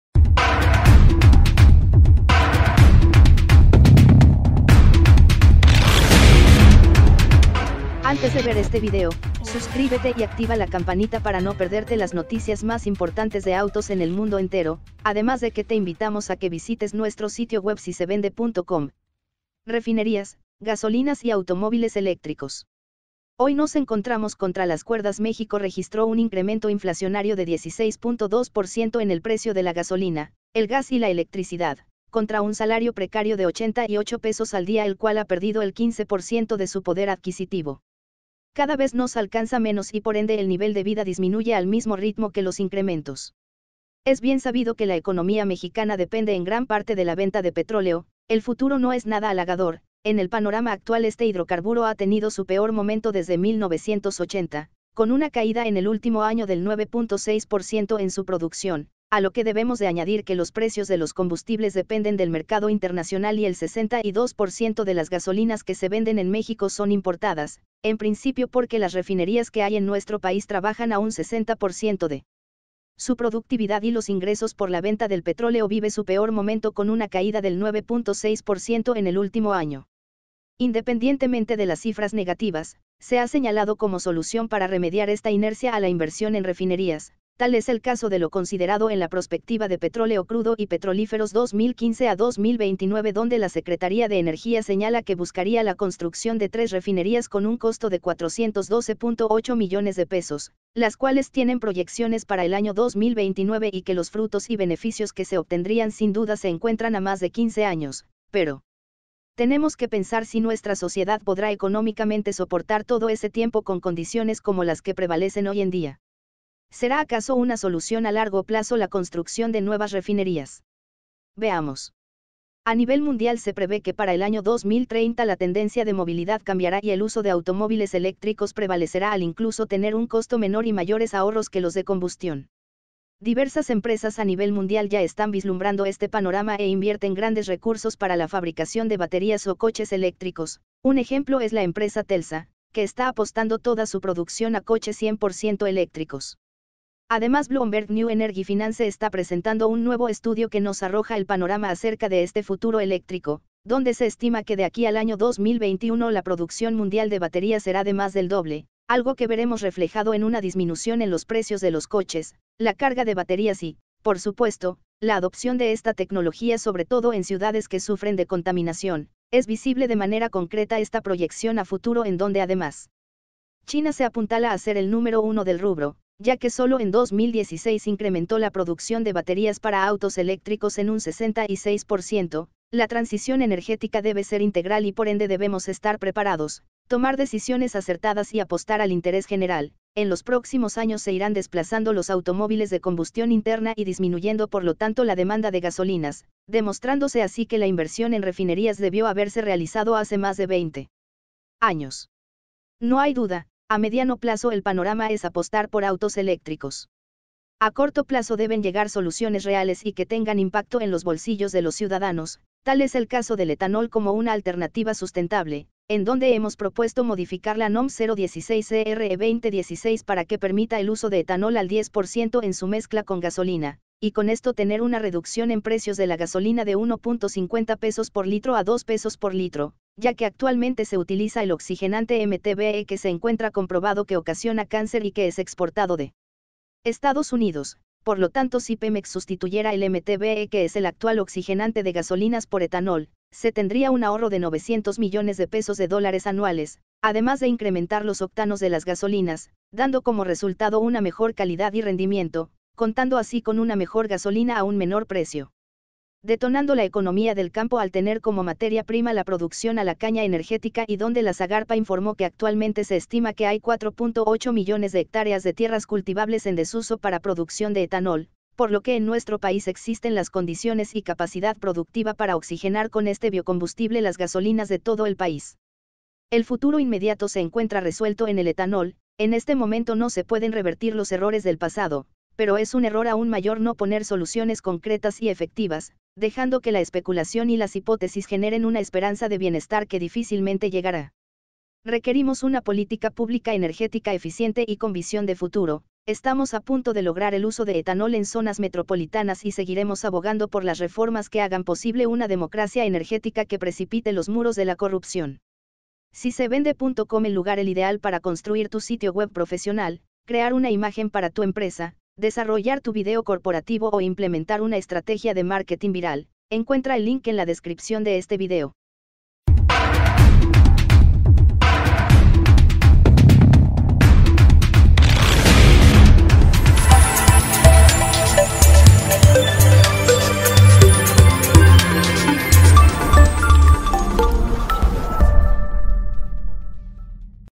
Refinerías, gasolinas y automóviles eléctricos.
Automóviles, vehículos, carros

Cursos de diseño web

Hoy nos encontramos "contra las cuerdas" México registró un incremento inflacionario de 16.2% en el precio de la gasolina, el gas y la electricidad, contra un salario precario de $88.00 al día el cual ha perdido el 15% de su poder adquisitivo. Cada vez nos alcanza menos y por ende el nivel de vida disminuye al mismo ritmo que los incrementos. Es bien sabido que la economía mexicana depende en gran parte de la venta de petróleo, el futuro no es nada halagador, en el panorama actual este hidrocarburo ha tenido su peor momento desde 1980, con una caída en el último año del 9.6% en su producción, a lo que debemos de añadir que los precios de los combustibles dependen del mercado internacional y el 62% de las gasolinas que se venden en México son importadas, en principio porque las refinerías que hay en nuestro país trabajan a un 60% de su productividad y los ingresos por la venta del petróleo vive su peor momento con una caída del 9.6% en el último año. Independientemente de las cifras negativas, se ha señalado como solución para remediar esta inercia a la inversión en refinerías, tal es el caso de lo considerado en la "Prospectiva de Petróleo Crudo y Petrolíferos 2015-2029" donde la Secretaría de Energía señala que buscaría la construcción de tres refinerías con un costo de 412.8 millones de pesos", las cuales tienen proyecciones para el año 2029 y que los frutos y beneficios que se obtendrían sin duda se encuentran a más de 15 años, pero tenemos que pensar si nuestra sociedad podrá económicamente soportar todo ese tiempo con condiciones como las que prevalecen hoy en día. ¿Será acaso una solución a largo plazo la construcción de nuevas refinerías? Veamos. A nivel mundial se prevé que para el año 2030 la tendencia de movilidad cambiará y el uso de automóviles eléctricos prevalecerá al incluso tener un costo menor y mayores ahorros que los de combustión. Diversas empresas a nivel mundial ya están vislumbrando este panorama e invierten grandes recursos para la fabricación de baterías o coches eléctricos, un ejemplo es la empresa Telsa, que está apostando toda su producción a coches 100% eléctricos. Además Bloomberg New Energy Finance está presentando un nuevo estudio que nos arroja el panorama acerca de este futuro eléctrico, donde se estima que de aquí al año 2021 la producción mundial de baterías será de más del doble, algo que veremos reflejado en una disminución en los precios de los coches, la carga de baterías y, por supuesto, la adopción de esta tecnología sobre todo en ciudades que sufren de contaminación, es visible de manera concreta esta proyección a futuro en donde además China se apuntala a ser el número uno del rubro, ya que solo en 2016 incrementó la producción de baterías para autos eléctricos en un 66%; la transición energética debe ser integral y por ende debemos estar preparados, tomar decisiones acertadas y apostar al interés general, en los próximos años se irán desplazando los automóviles de combustión interna y disminuyendo por lo tanto la demanda de gasolinas, demostrándose así que la inversión en refinerías debió haberse realizado hace más de 20 años. No hay duda, a mediano plazo el panorama es apostar por autos eléctricos.